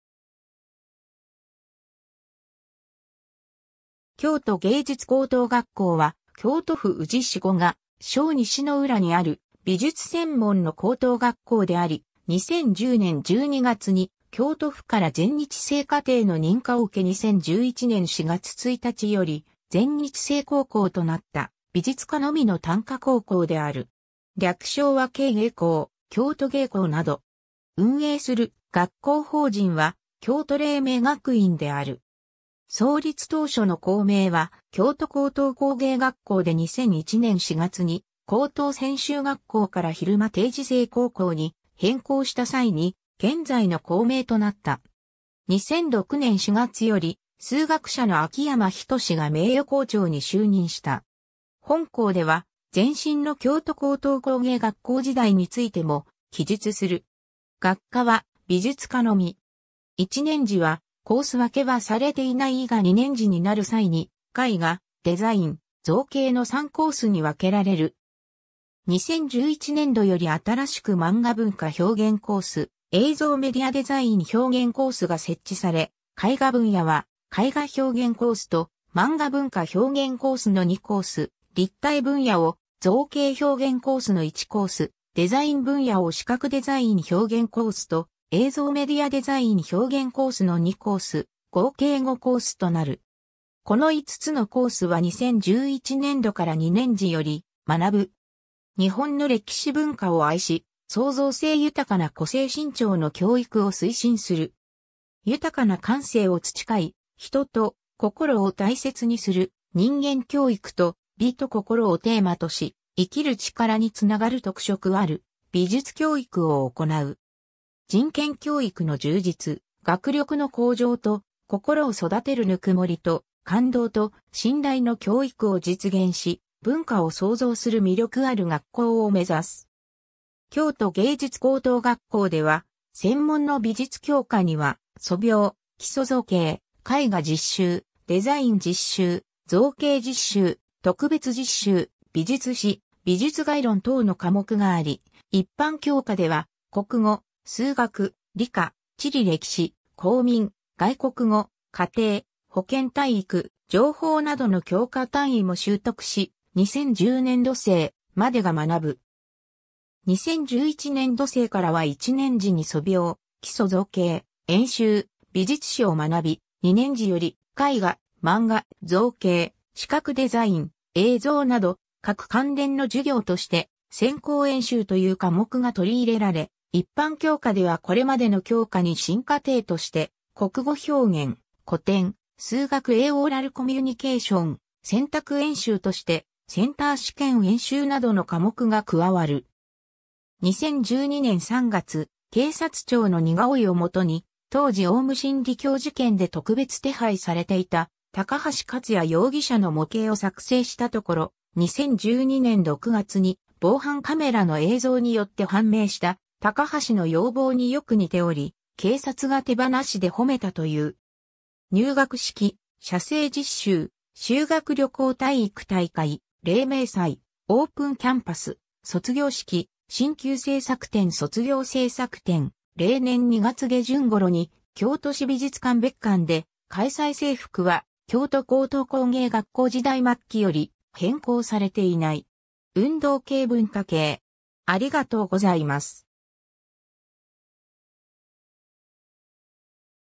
京都芸術高等学校
京都芸術高等学校（きょうとげいじゅつこうとうがっこう）は、京都府宇治市五ヶ庄西浦にある美術専門の高等学校であり、2010年12月に京都府から全日制課程の認可を受け2011年4月1日より全日制高校となった美術科のみの単科高校である（2010年度までは昼間定時制高校）。
 略称は京芸高、京都芸高など。
 運営する学校法人は京都黎明学院である。
 創立当初の校名は京都高等工芸学校（きょうとこうとうこうげいがっこう）で2001年4月に高等専修学校から昼間定時制高校に変更した際に現在の校名となった。
 2006年4月より数学者の秋山仁が名誉校長に就任した。
 本項では前身の京都高等工芸学校時代に付いても記述する。
 学科は美術科のみ。
1年次はコース分けはされていないが2年次になる際に絵画・デザイン・造形の3コースに分けられる（2009年度生までは卒業まで3つのコースで学ぶ）。
2011年度より新しくマンガ文化表現コース・映像メディアデザイン表現コースが設置され、絵画分野は絵画表現コースとマンガ文化表現コースの2コース、立体分野を造形表現コースの1コース、デザイン分野を視覚デザイン表現コースと映像メディアデザイン表現コースの2コース、合計5コースとなる。
この5つのコースは2011年度から2年次より学ぶ（2010年度生から適用される）。
 日本の歴史文化を愛し、創造性豊かな個性伸長の教育を推進する。
豊かな感性を培い「人と心」を大切にする人間教育と、美と心をテーマとし「生きる力」につながる特色ある美術教育を行う。
「人権教育の充実、学力の向上」と「心を育てる　ぬくもりと、感動と、信頼」の教育を実現し、文化を創造する魅力ある学校を目指す。
 京都芸術高等学校では専門の美術教科には素描、基礎造形、絵画実習、デザイン実習、造形実習、特別実習、美術史、美術概論等の科目があり一般教科では国語・数学・理科・地理歴史・公民・外国語・家庭・保健体育・情報などの教科単位も修得し、2010年度生までが学ぶ。
2011年度生からは1年次に素描、基礎造形、演習、美術史を学び、2年次より絵画、マンガ、造形、視覚デザイン、映像など各関連の授業として専攻演習という科目が採り入れられ、一般教科ではこれまでの教科に新課程として国語表現・古典・数学A・オーラルコミュニケーション、選択演習としてセンター試験演習などの科目が加わる。
 2012年3月、警察庁の似顔絵を元に、当時オウム真理教事件で特別手配されていた高橋克也容疑者（当時。現被告）の模型を作成したところ、2012年6月に防犯カメラの映像によって判明した高橋の容貌に良く似ており、警察が手放しで褒めたという。
 入学式、写生実習、修学旅行（写生研修旅行）（6月に北海道方面で実施） 体育大会、黎明祭（文化祭）、オープンキャンパス、卒業式、進級制作展（1、2年生）卒業制作展（3年生） 例年2月下旬頃に京都市美術館別館で開催 制服は京都高等工芸学校時代末期より変更されていない。
 運動系 文化系